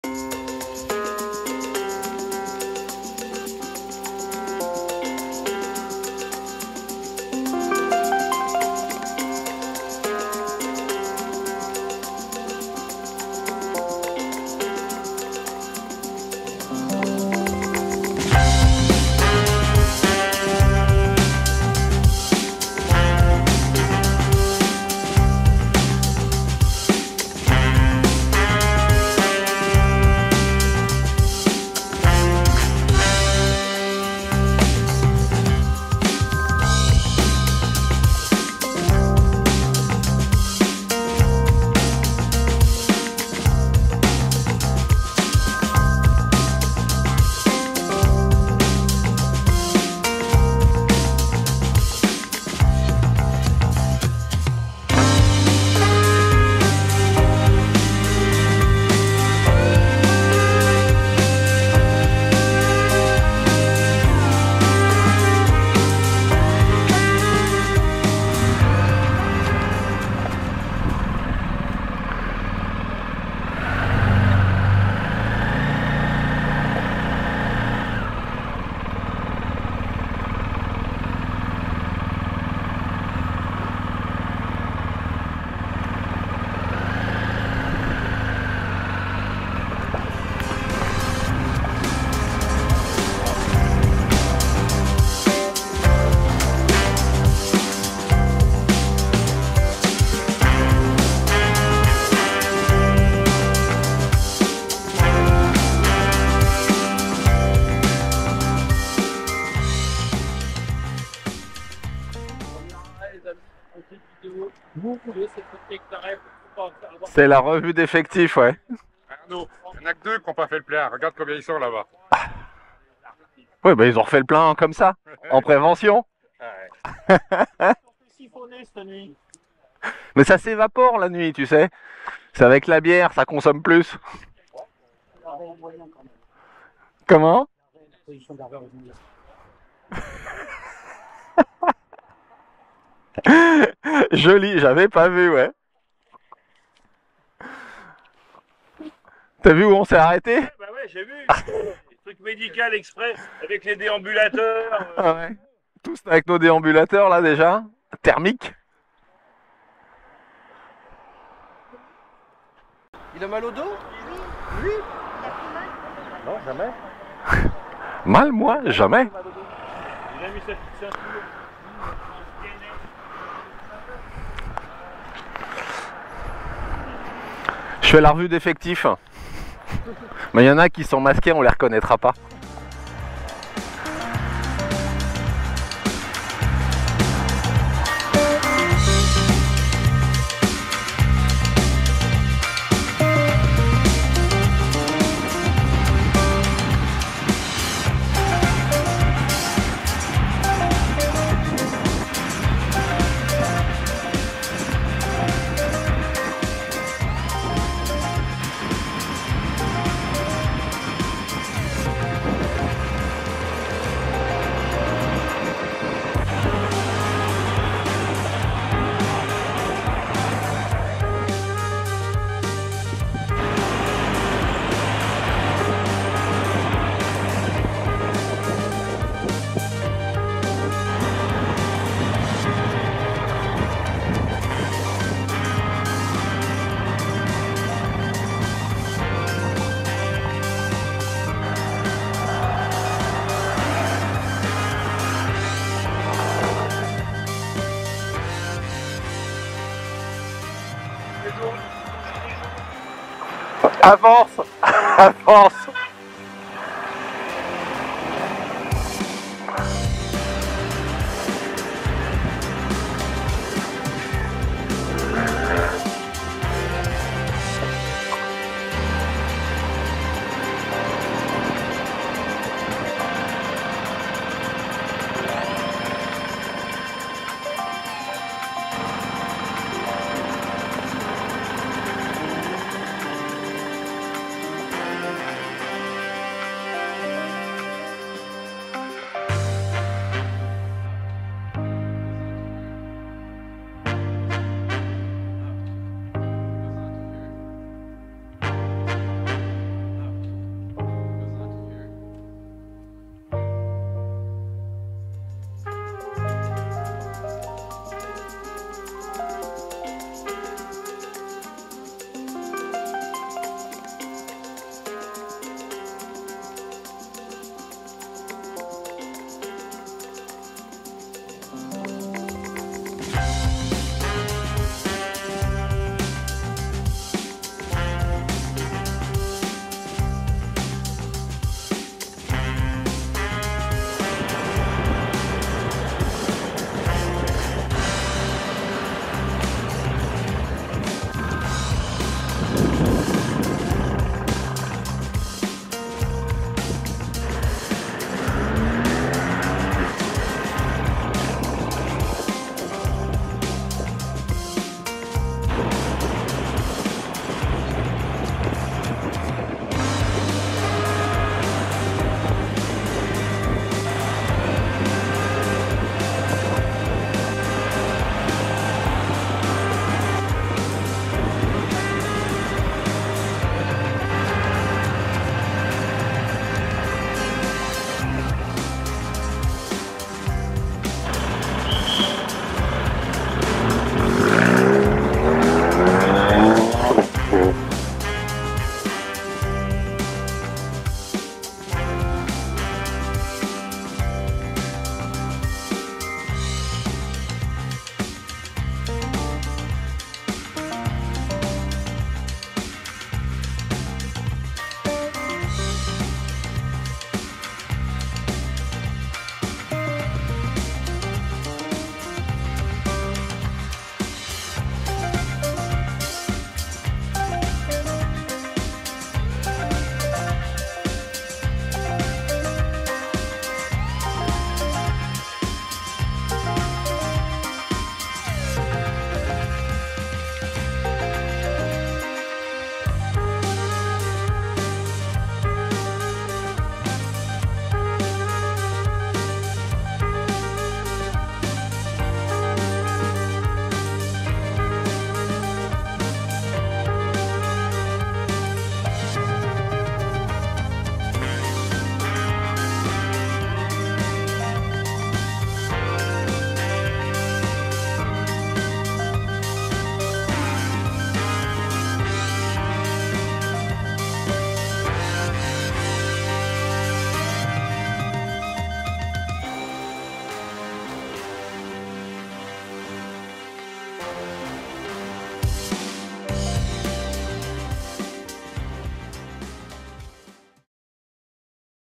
SGT3R : Rasso 2023 (partie 1)
Rasso 2023 des membres du forum SGT3R dans les Ardennes le 28 Mai 2023
Circuit au départ de Charleville-Mézières
Partie 1: départ de Charleville-Mézières, Point de vue de l’Hermitage, statue des 4 fils Aymon